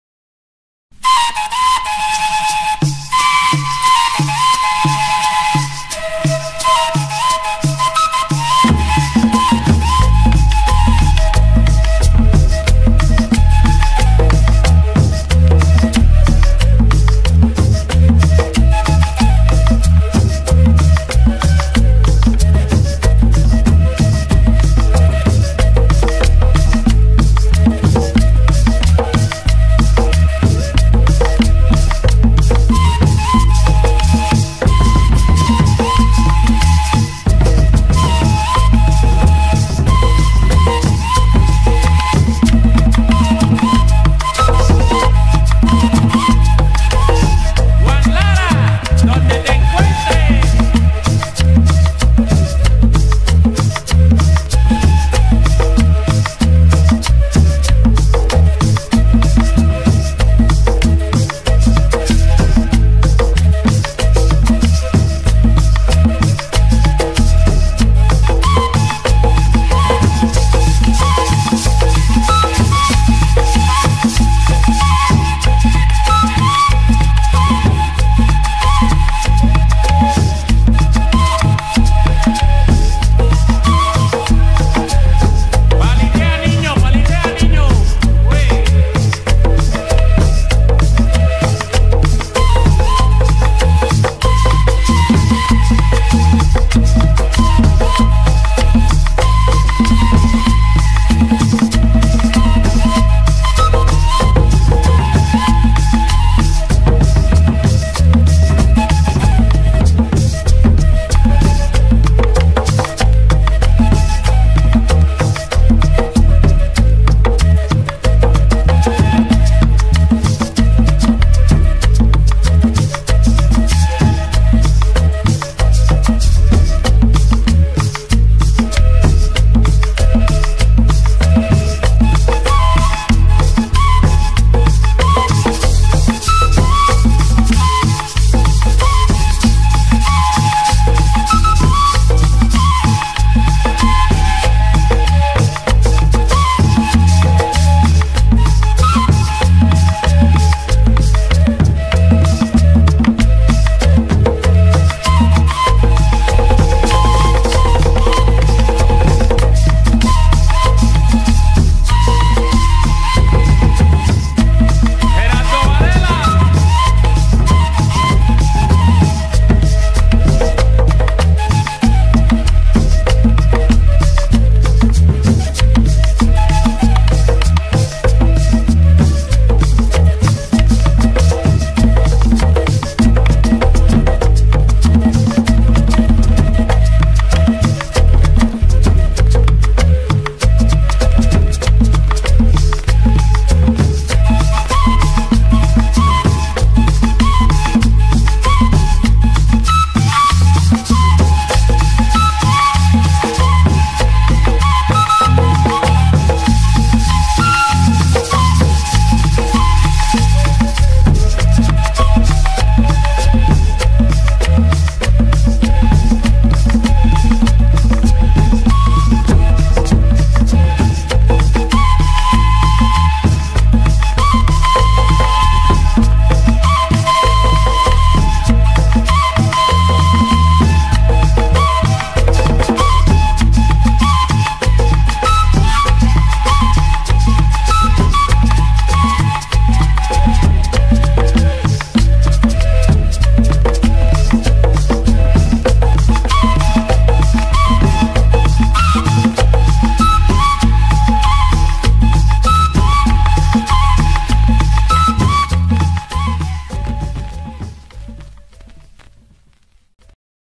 🚩UNA VERDADERA CUMBIA SONIDERA (((HOMENAJE A JUAN LARA))) RICHARD TV EN COLOMBIA
🚩UNA VERDADERA CUMBIA SONIDERA (((HOMENAJE A JUAN LARA))) RICHARD TV EN COLOMBIA 🚩AHORA AMIGOS LESDEJO UNA CUMBIA NACIDA EN COLOMBIA CON UNO DE LOS GRANDES DE LA CUMBIA GERRARDO VARELA Y RICHARD TV RECORRIENDO CARTAGENA DE INDIAS COLOMBIA. 🔊🚩 SALUDO PUBLICO..!!! ATT RICHARD TV.✌ ↓↓↓↓↓↓↓↓↓DALE CLIK..!!

🔊🚩 DILES QUE ERES FAN DE RICHARD TV